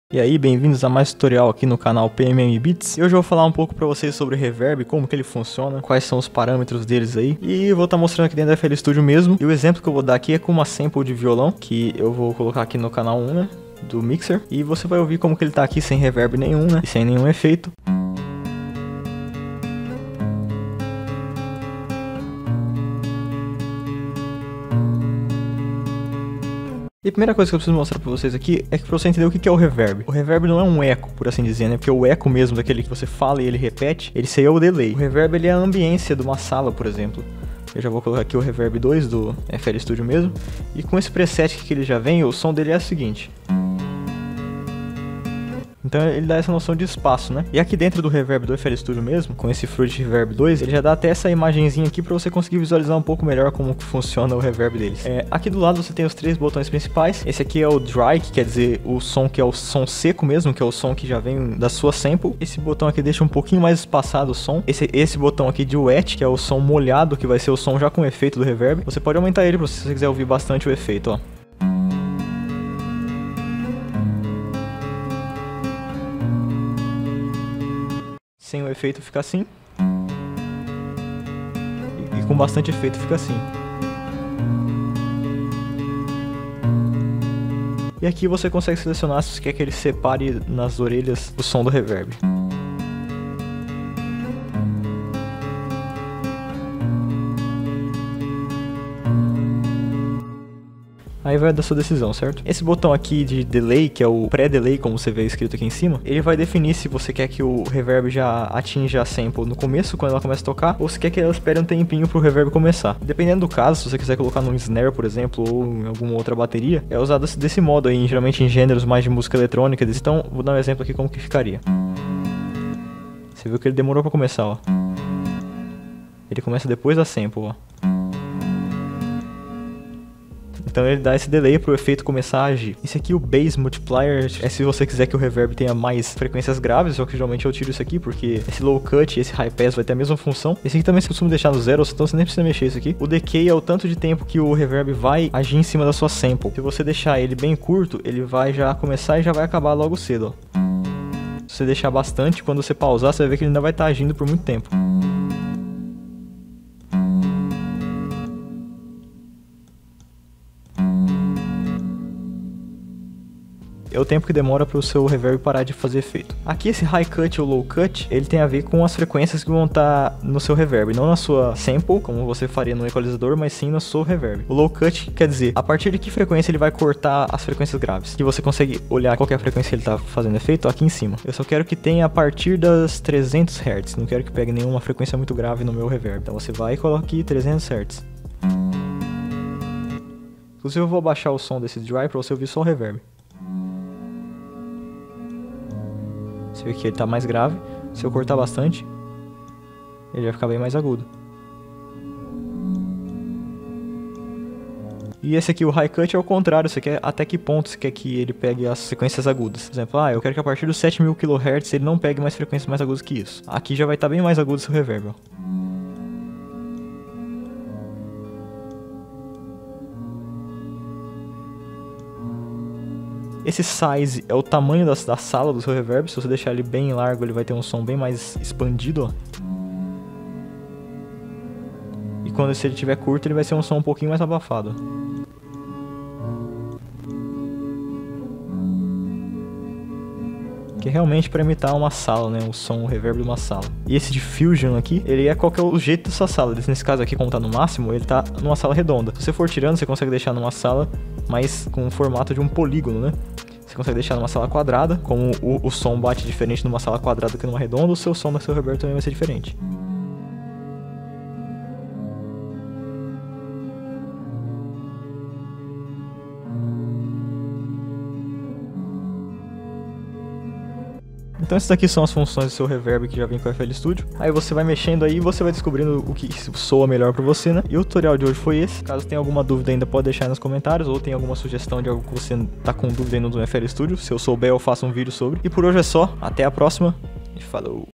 Reverb - Mixagem Pt. 2 (Tutorial FL Studio #10)
Conversando sobre como funciona o reverb na produção musical, mais um dos plugins mais importantes para mixagem e masterização, tudo pelo FL Studio!!!

Meus pacotes de bateria, loops e afins:
› Downloads gratuitos (atualizados com frequência):

● Quer escutar mais da minha música e/ou ter um beat meu em sua música?